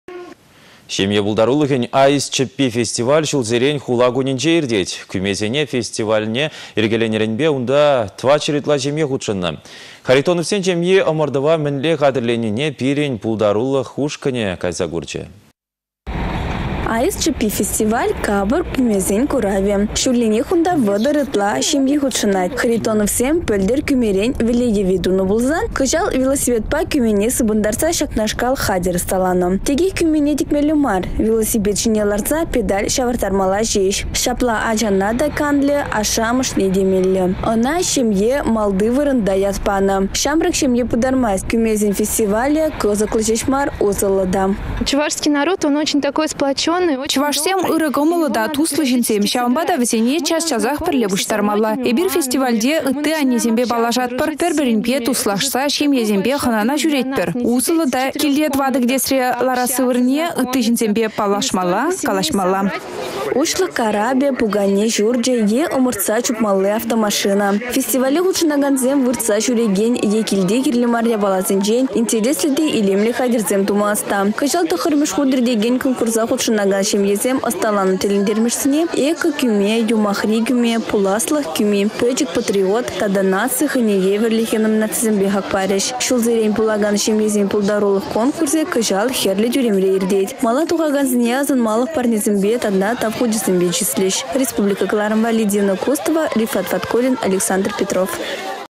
«Аист чĕппи» фестивале валли илемлетнĕ кÿме хатĕр
Çемье пултарулăхĕн «Аист чĕппи» фестиваль çулсерен хула кунĕнче иртет. Кÿмесене фестивальне йĕркеленнĕренпе унта 400 ытла çемье хутшăннă. Харитоновсен çемьи ăмăртăва мĕнле хатĕрленнине пирĕн пултарулăх ушкăнĕ кайса курчĕ.